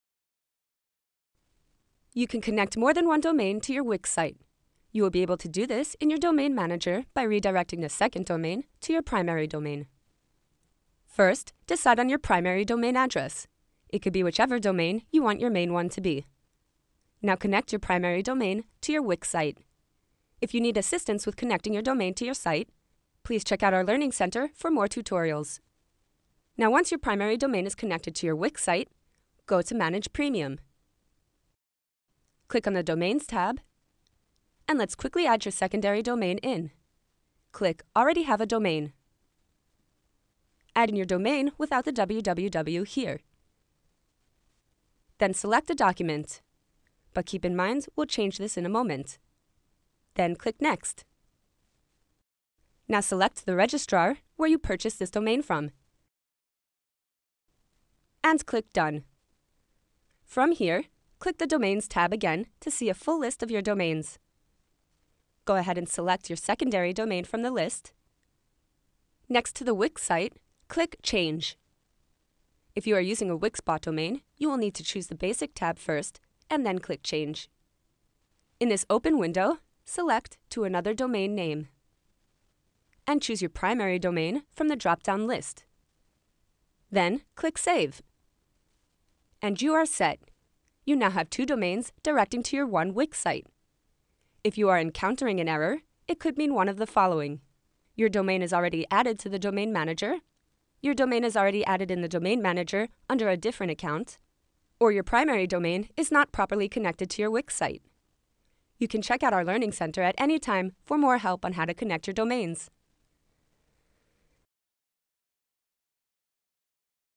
Connect more than 1 domain to your Wix Site
In this video you will learn how to connect more than 1 domain to your Wix website. You can point your secondary domain to your primary domain inside Wix's Domain Manager. You can save money designing your own website using Wix.com and get your business online today! Click the link below to sign up for free!